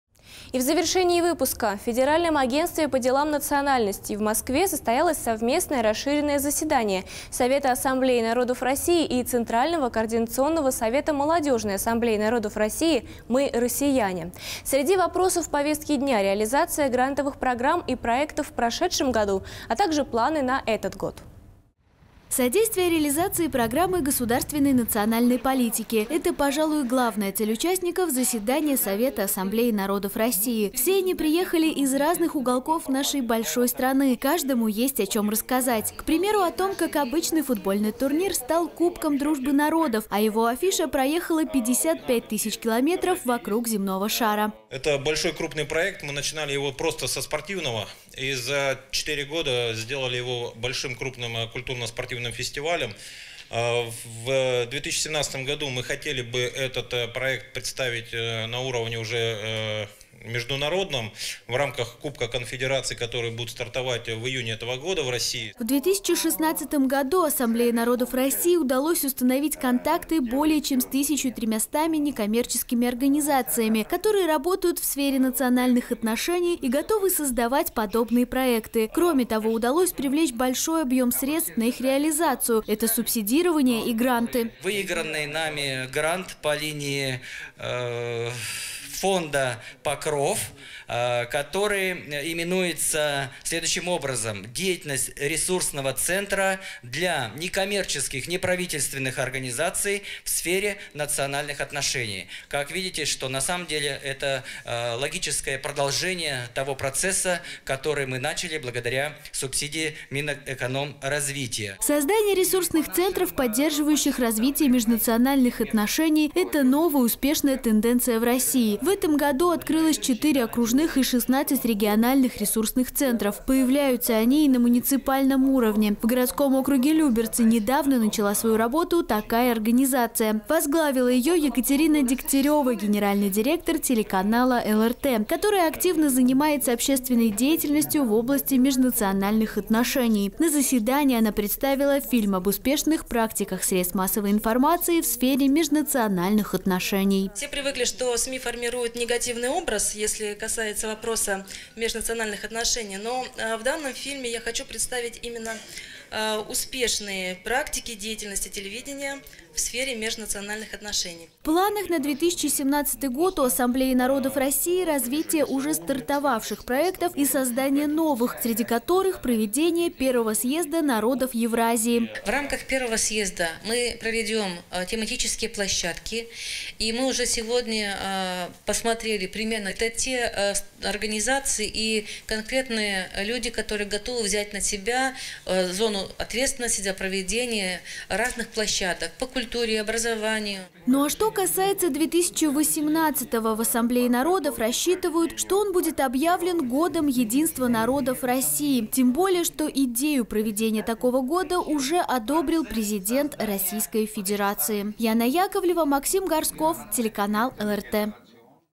Прошло расширенное заседание Совета Ассамблеи народов России
В Федеральном агентстве по делам национальностей в Москве состоялось совместное расширенное заседание Совета Ассамблеи народов России и Центрального Координационного Совета Молодежной Ассамблеи народов России «МЫ – РОССИЯНЕ». Среди вопросов повестки дня – реализация грантовых программ и проектов в прошедшем году и планы на этот год.

Содействие реализации  стратегии государственной национальной политики – это, пожалуй, главная цель участников заседания Совета Ассамблеи народов России. Все они приехали из разных уголков нашей большой страны. Каждому есть о чем рассказать. К примеру, о том, как обычный футбольный турнир стал «Кубком дружбы народов», а его афиша проехала 55 тысяч километров вокруг земного шара.

Юрий Стародубцев, руководитель Карельской региональной общественной организации «Спортивная инициатива»:
"Это большой крупный проект, мы начинали его просто со спортивного и за 4 года сделали его большим крупным культурно-спортивным фестивалем. В 2017 году мы хотели бы этот проект представить на уровне уже международном, в рамках кубка конфедерации, который будет стартовать в июне этого года в России".

В 2016 году Ассамблеи народов России удалось установить контакты более чем с 1300 некоммерческими организациями, которые работают в сфере национальных отношений и готовы создавать подобные проекты. Кроме того удалось привлечь большой объем средств на их реализацию – это субсидирование и гранты.

Александр Плотников,  директор Ресурсного центра Ассамблеи народов России:
"Выигранный нами грант по линии фонда «Покров», который именуется следующим образом: «Деятельность ресурсного центра для некоммерческих неправительственных организаций в сфере национальных отношений». Как видите, что это логическое продолжение того процесса, который мы начали благодаря субсидии «Минэкономразвития»".

Создание ресурсных центров, поддерживающих развитие межнациональных отношений – это новая успешная тенденция в России. В этом году открылось 4 окружных и 16 региональных ресурсных центров. Появляются они и на муниципальном уровне. В городском округе Люберцы недавно начала свою работу такая организация. Возглавила ее Екатерина Дегтярева, генеральный директор телеканала ЛРТ, которая активно занимается общественной деятельностью в области межнациональных отношений. На заседании она представила фильм об успешных практиках средств массовой информации в сфере межнациональных отношений.

Екатерина Дегтярева, генеральный директор ООО «ЛРТ», руководитель Ресурсного центра МО АНР:
"Все привыкли, что СМИ формирует негативный образ, если касается вопроса межнациональных отношений. Но в данном фильме, я хочу представить именно успешные практики  деятельности телевидения в сфере межнациональных отношений".

В планах на 2017 год у Ассамблеи народов России – развитие уже стартовавших проектов и создание новых. Среди которых – проведение Первого Съезда народов Евразии.

Светлана Смирнова, председатель Совета Ассамблеи народов России:
"В рамках первого съезда мы проведем тематические площадки. И мы уже сегодня посмотрели примерно. Это те организации и конкретные люди, которые готовы взять на себя зону ответственности за организацию разных площадок: по культуре, образованию".

Ну, а что касается 2018. В Ассамблее народов рассчитывают, что он будет объявлен Годом единства народов России. Тем более, что идею проведения такого года уже одобрил президент Российской Федерации.